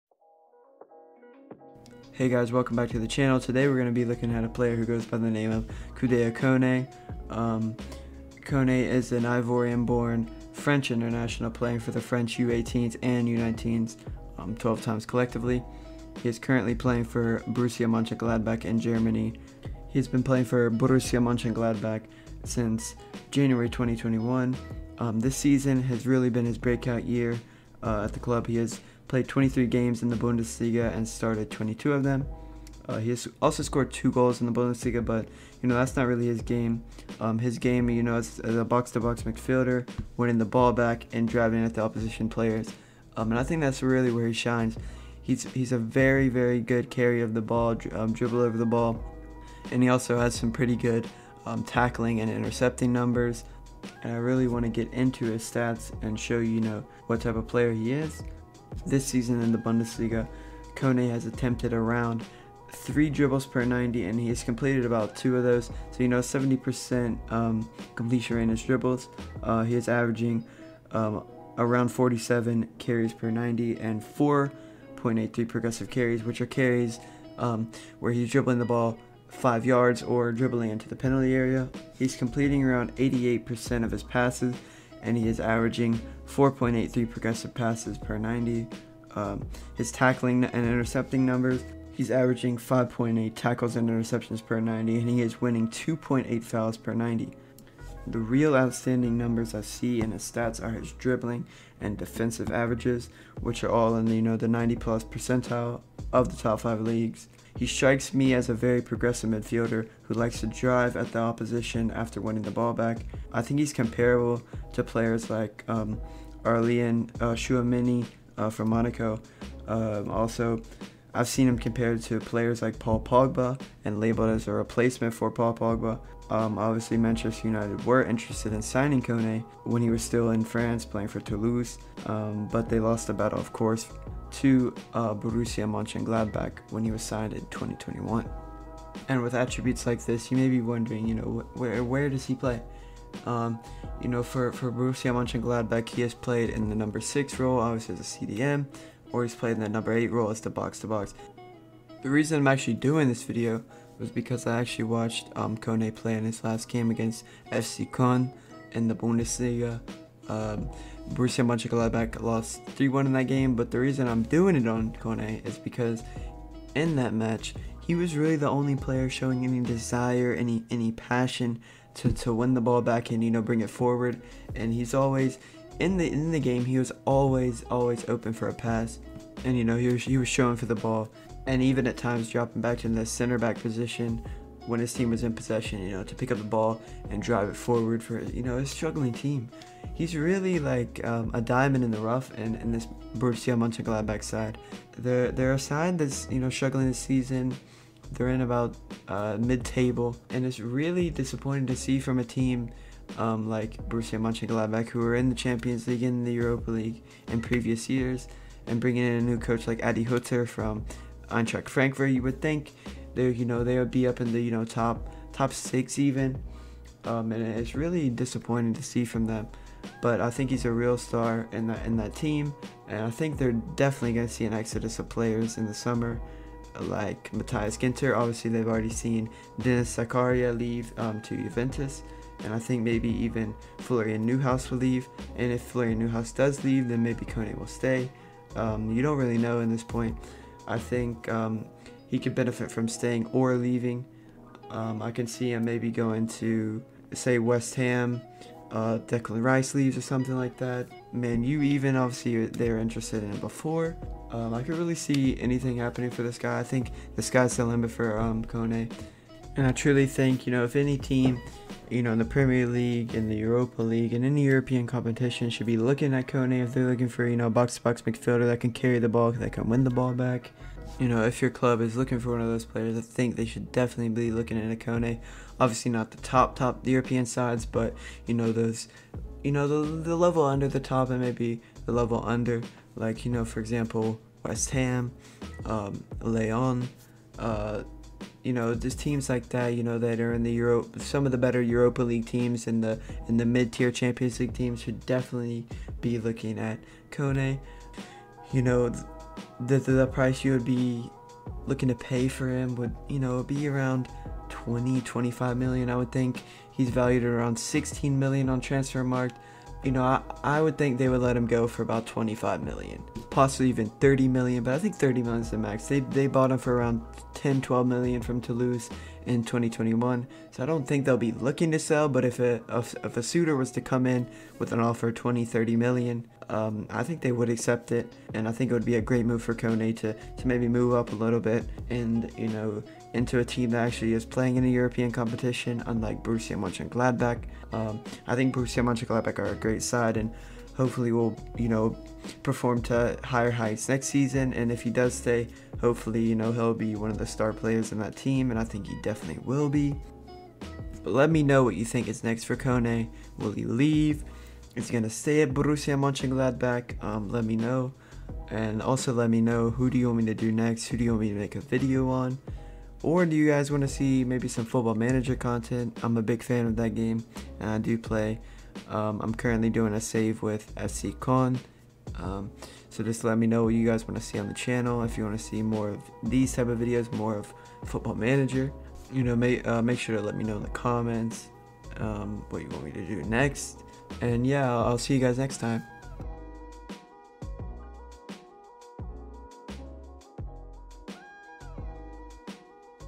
The Next French Starlet? Who is Kouadio Koné?!
In this video we are going to be looking at promising Borussia Mönchengladbach midfielder Kouadio Koné

This video is intended for entertainment and educational purposes.